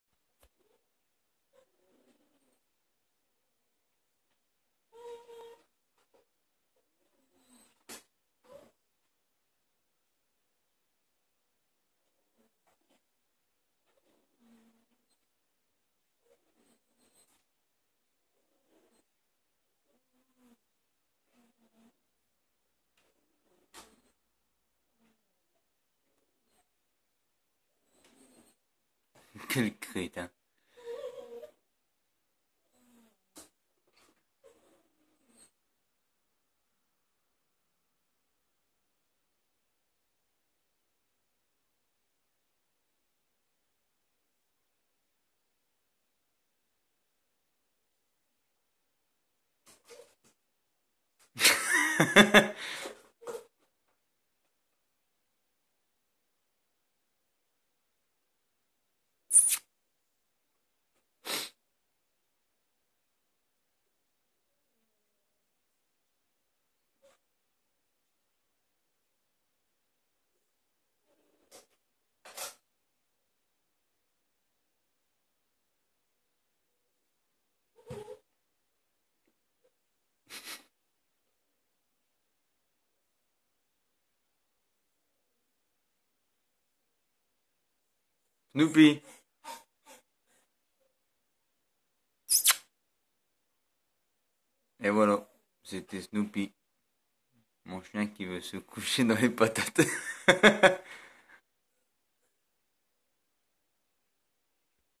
Mon chien qui veut se coucher dans les patates
A mourir de rire se snoopy